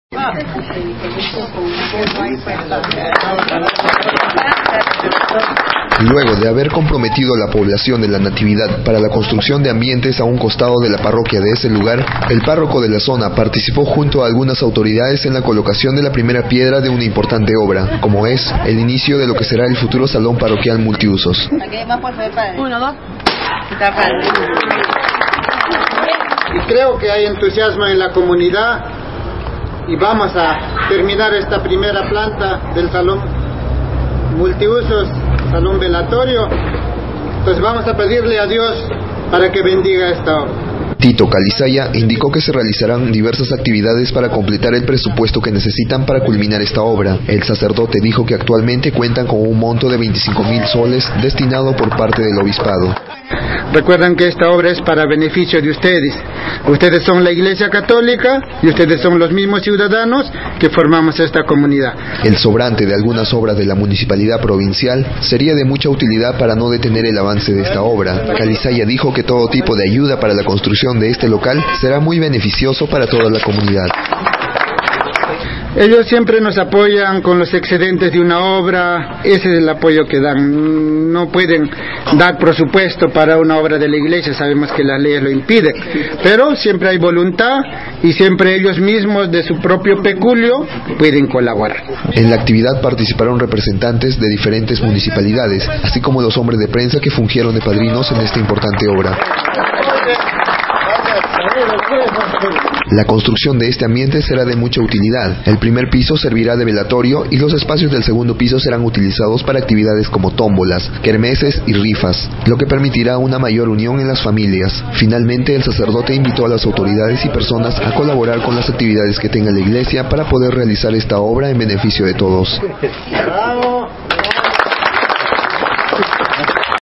PÁRROCO PIDE APOYO PARA CONSTRUIR LOCAL MULTIUSOS
El párroco de la zona participo junto a algunas autoridades en La colocación de la primera piedra de una importante obra.